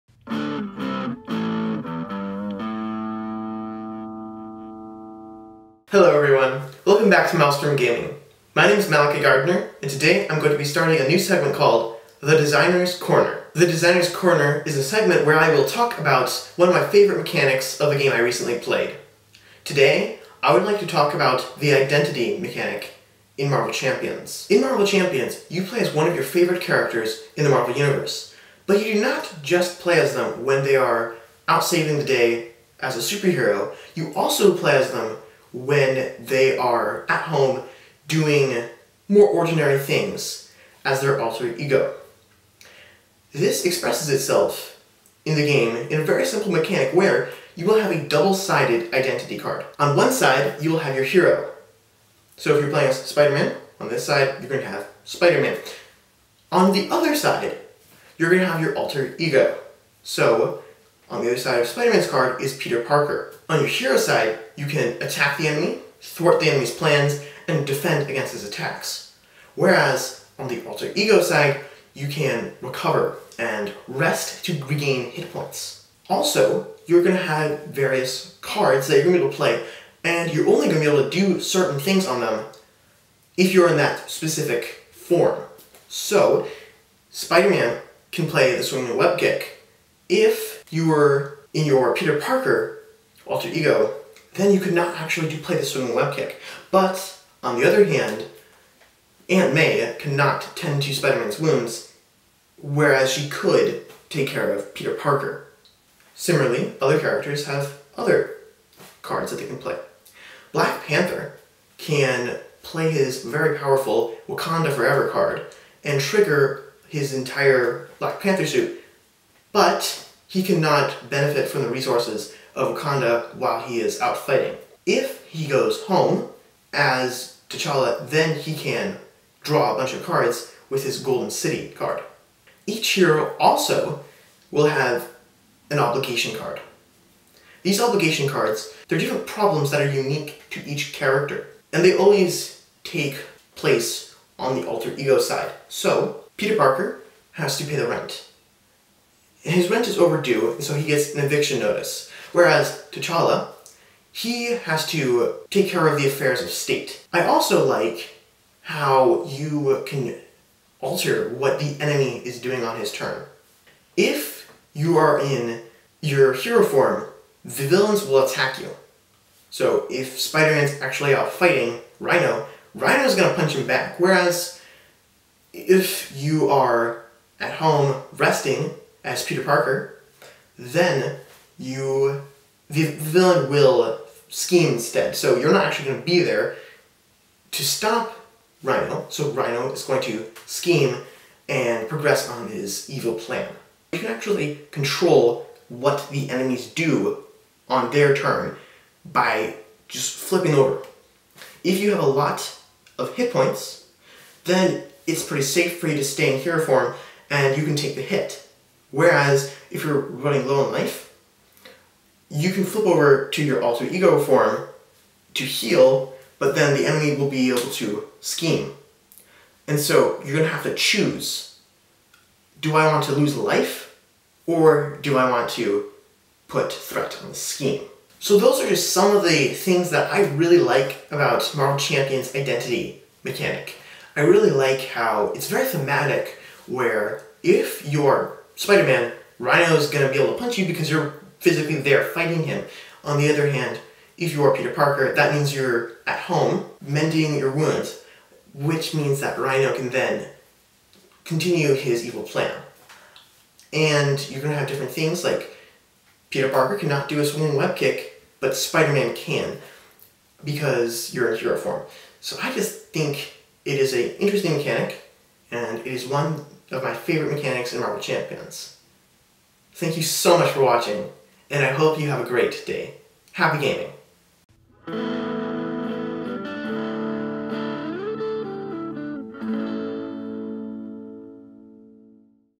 Designer's Corner Identity
Today I look at one of my favorite mechanics in Marvel Champions, your character's identity.